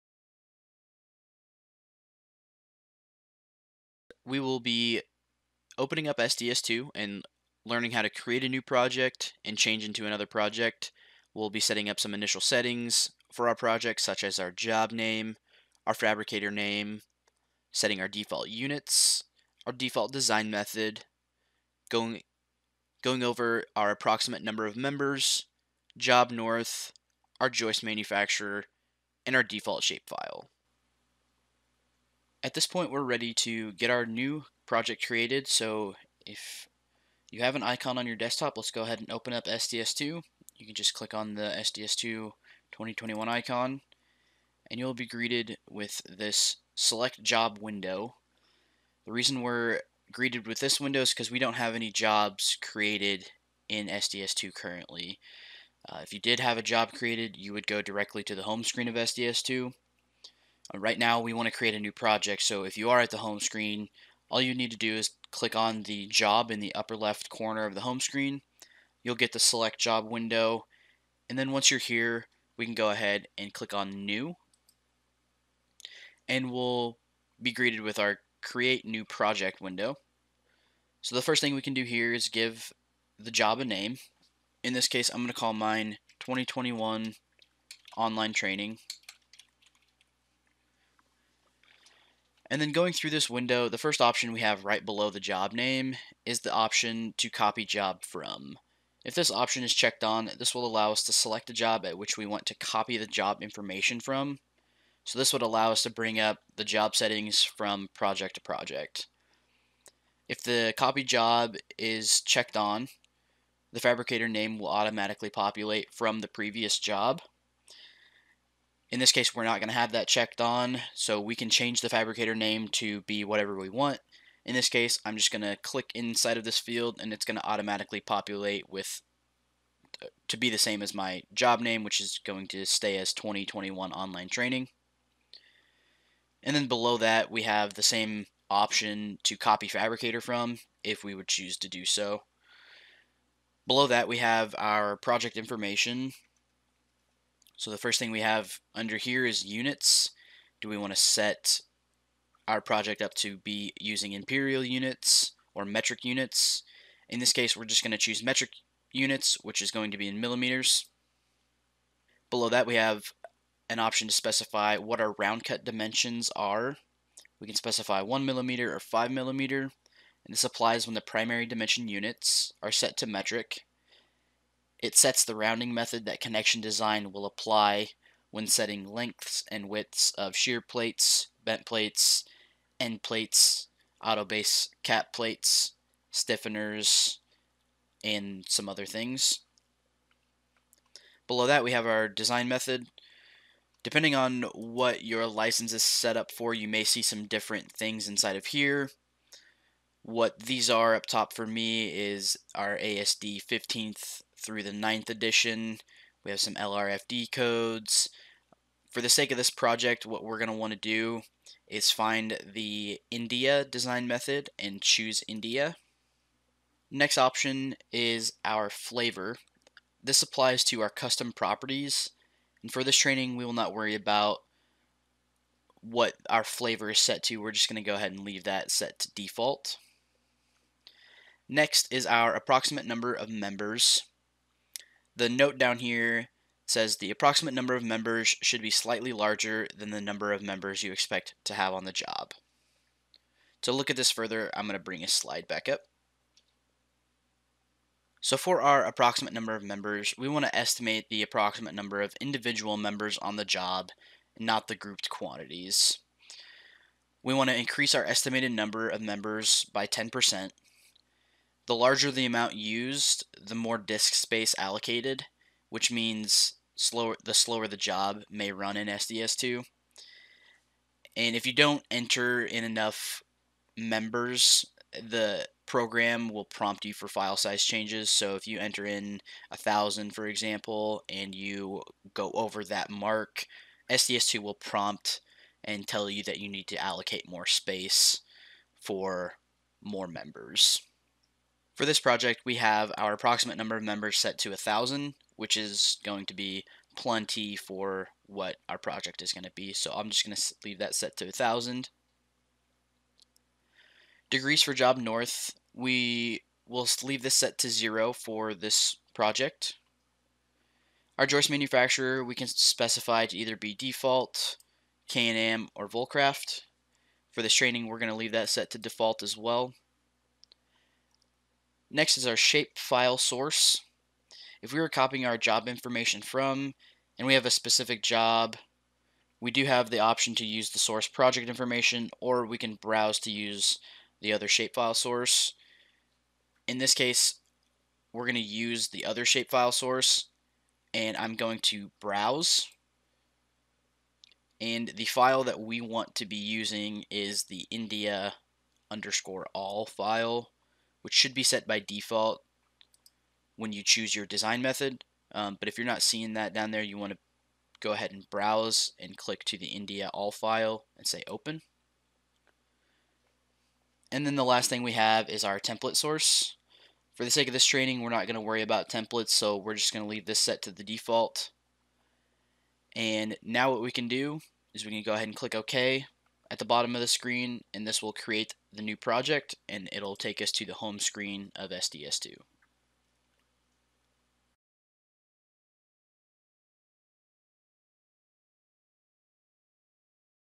Getting Started: How to Create a Project in SDS2 2021 India
In this video, you will learn how to create a project from scratch in SDS2 2021.